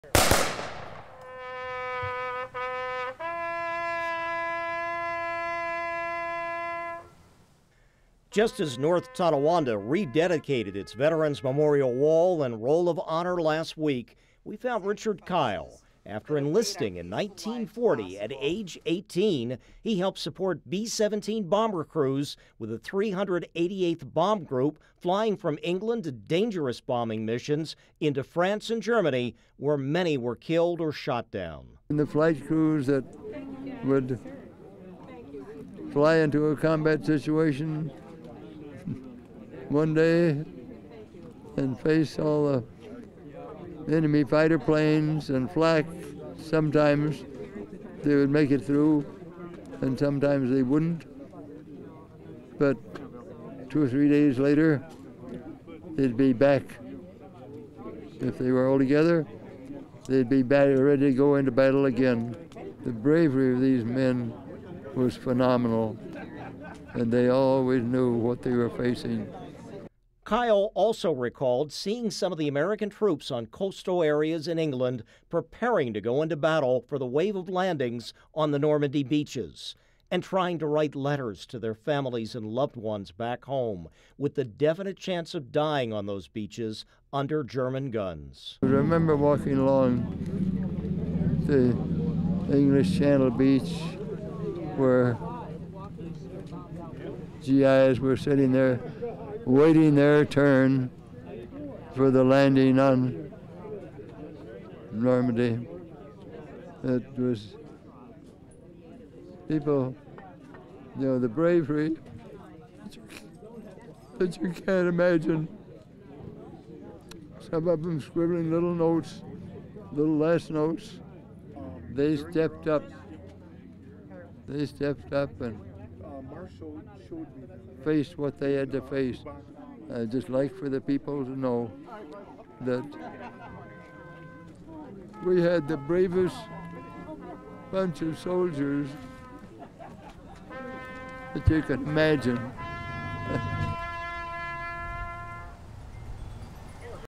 Remembering D-Day, 80 years later
Richard Kyle, a North Tonawanda man who is 102 years old, served with the Army Air Corps as a ground support crew member for B-17 bomber crews.